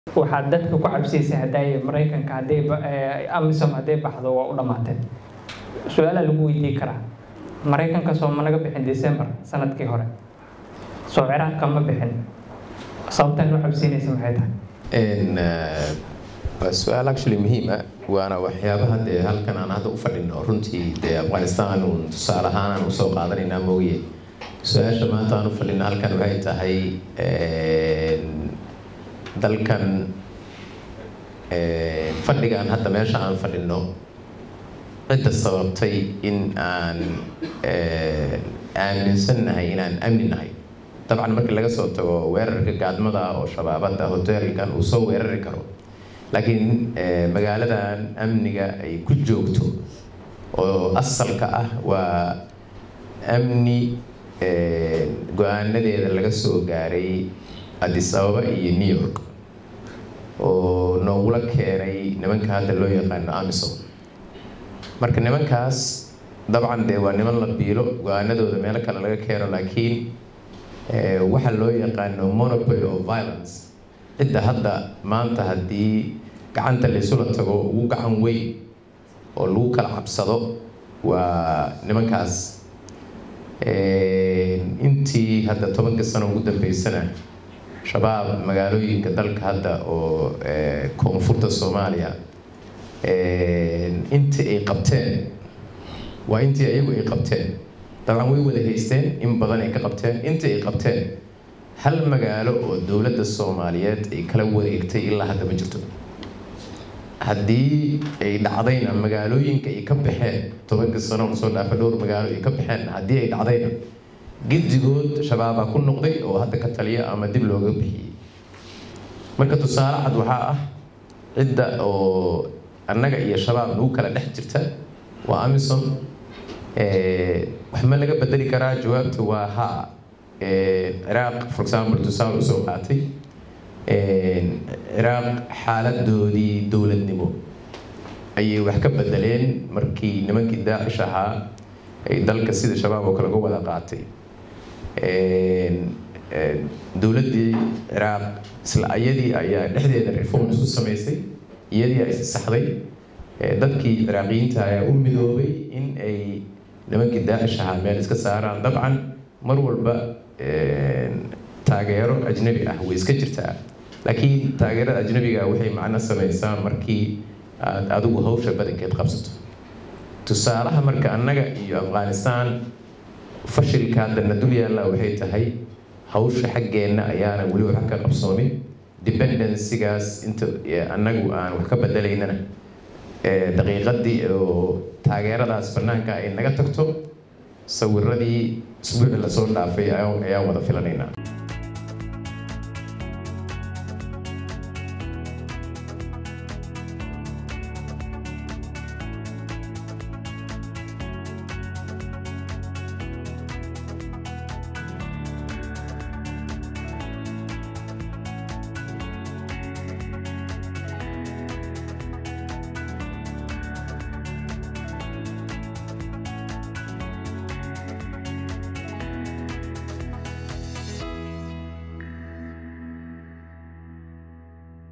Xuseen Macallin “Magaalada Muqdisho Amniga lagu joogo waxaa lagu garaa New York iyo Addis Ababa”
Xuseen Macallin Maxamuud “Magaalada Muqdisho Amniga lagu joogo waxaa lagu garaa New York iyo Addis Ababa”